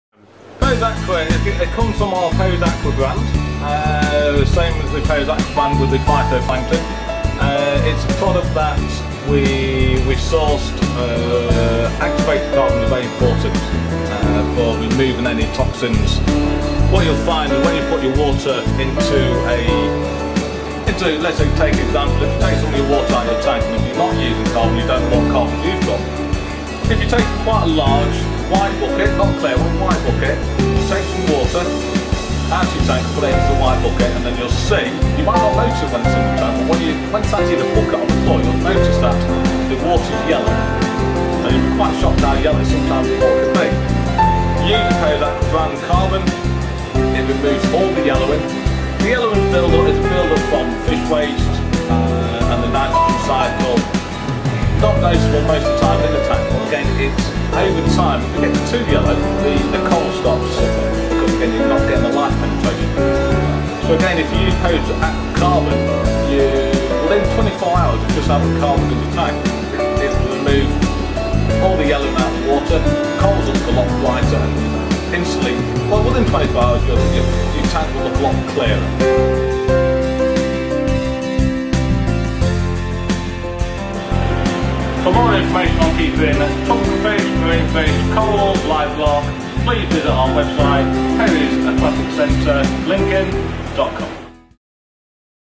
Activated Carbon for Aquariums
Perry's Aqua Brand Activated Carbon is all you need to keep your aquarium clean and healthy for marine fish, tropical fish, gold fish, live rock and corals.
Can be used in external and internal filters.
200g treats up to 1000 litres of water.
Carbon should be replaced every 6-8 weeks depending on load.